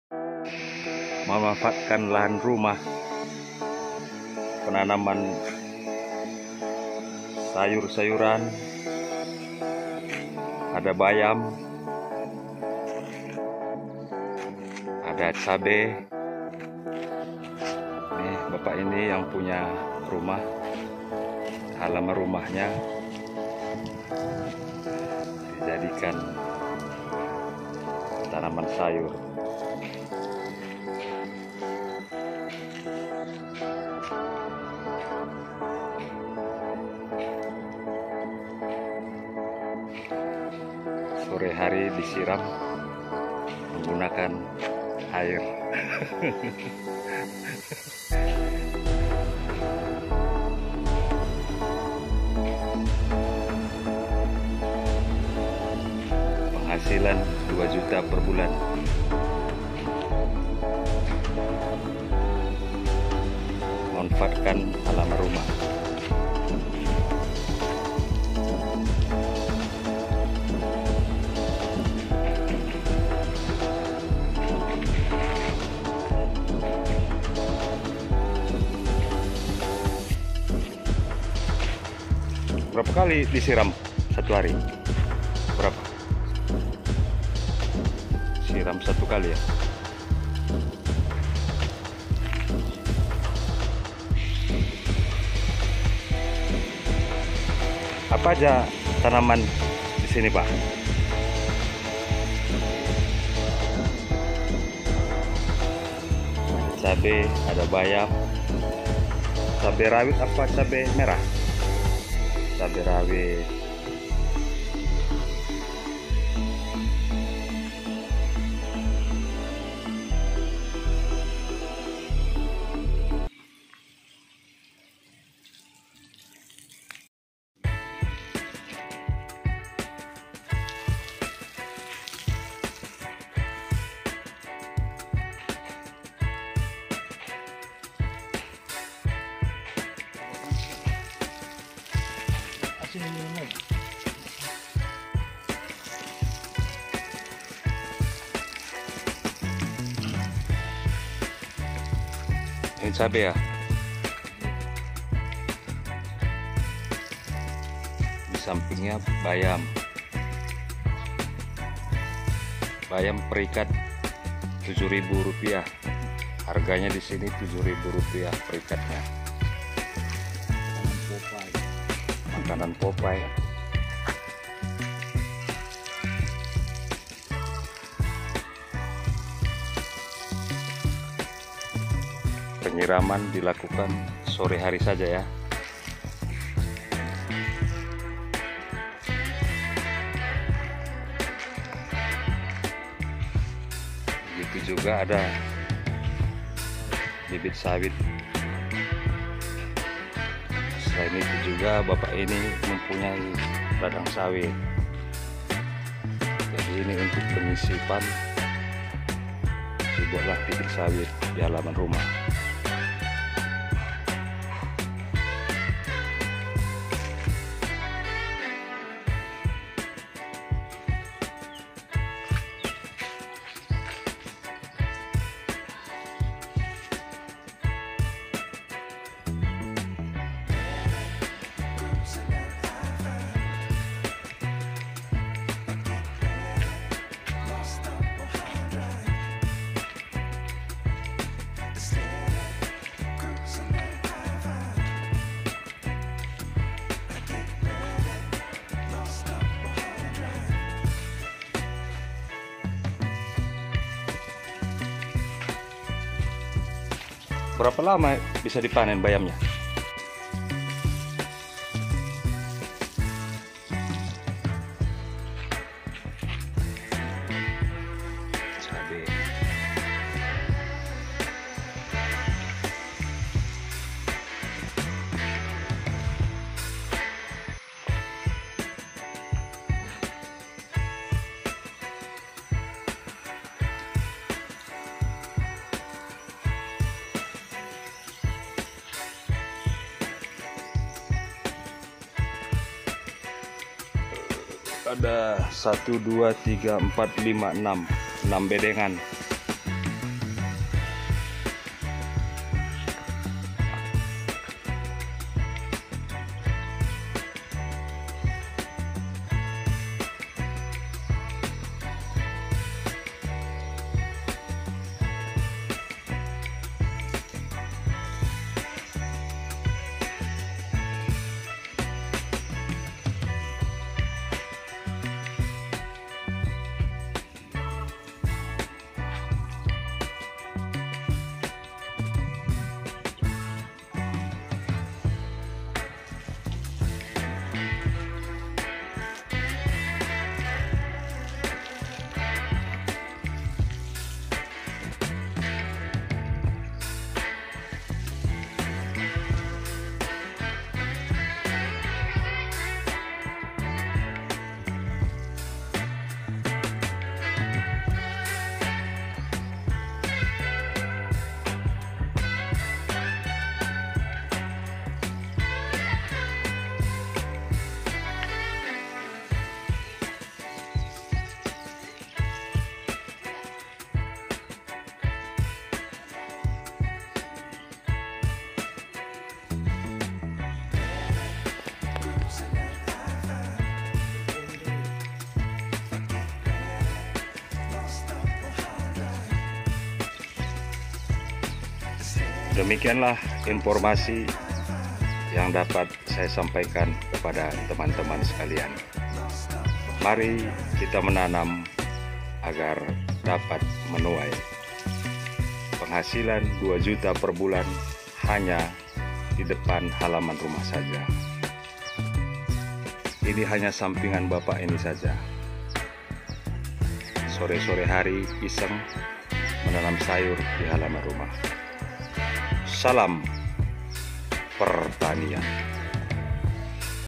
menanam sayur di halaman rumah yang sempit
petani ini menanam sayur dihalaman rumahnya
yang sempit sangat menginspirasi dan
menanam sayur di pekarangan menambah penghasilan, bapak ini tidak membiarkan halaman rumahnya kosong begitu saja, ia menanam sayur secara organik dengan memakai pupuk kompos dan pestisida alami sehingga aman dikonsumsi.